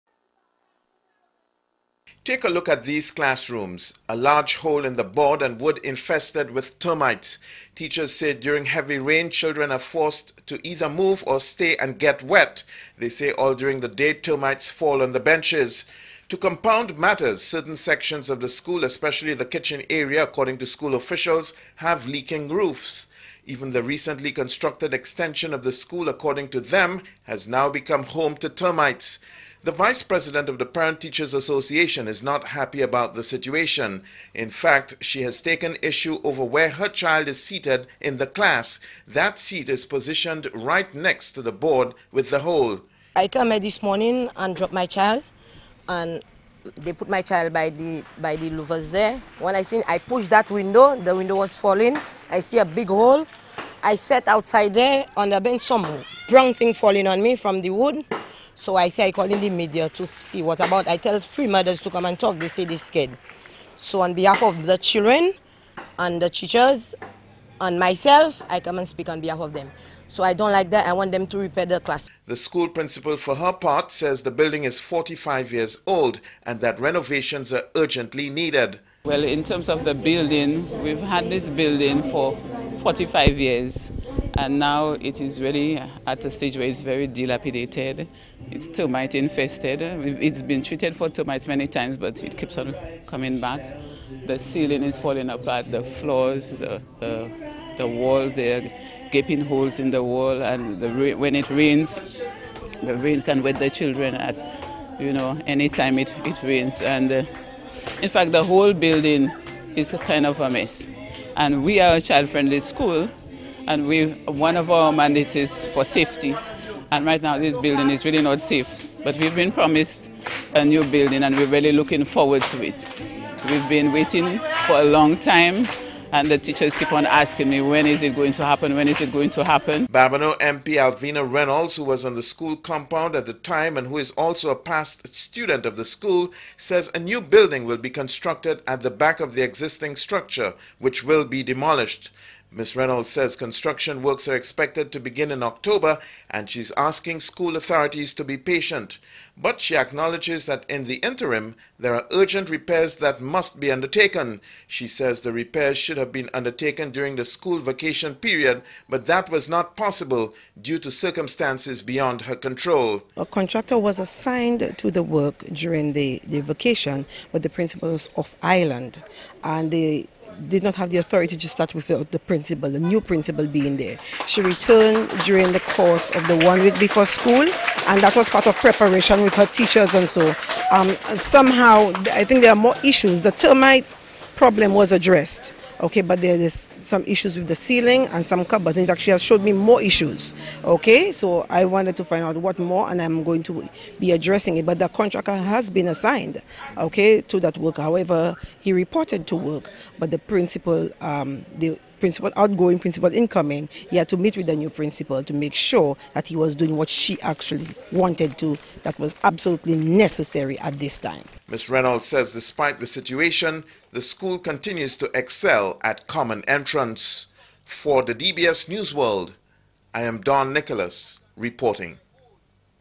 recondition
Parents and teachers at the Fond Assau combined  school have expressed concern about the conditions at the institution.
DBS' Don Nicholas  tells us more: